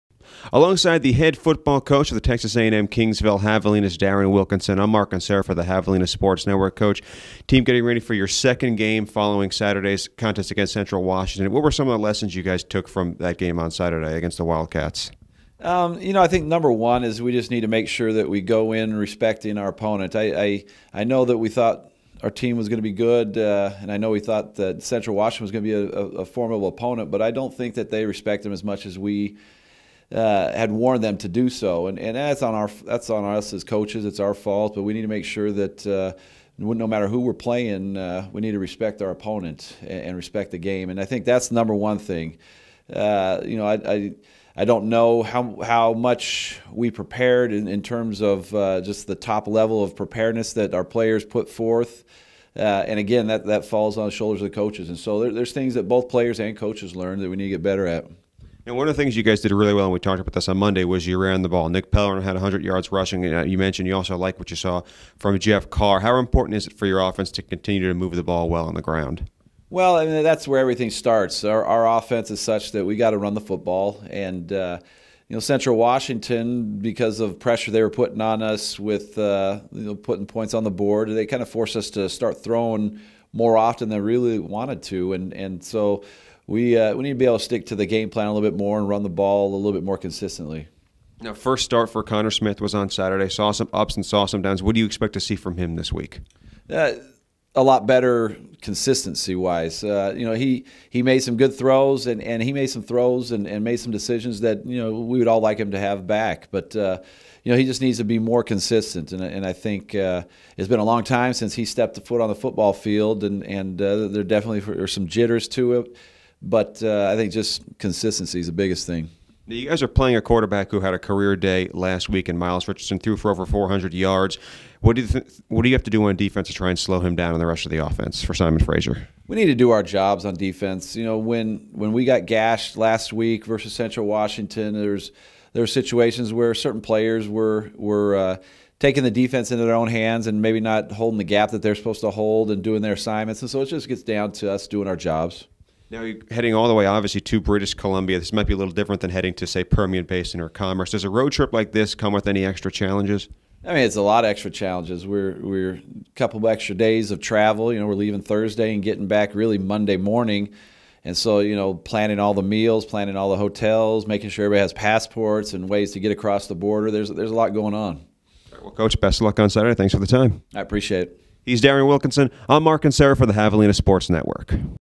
Javelina Football: Daren Wilkinson Interview 9-8-2017
Coach Wilkinson previews his team's road clash with the Simon Fraser Clan on September 9th.

Fans are also encouraged to follow and interact on all Javelina social media accounts.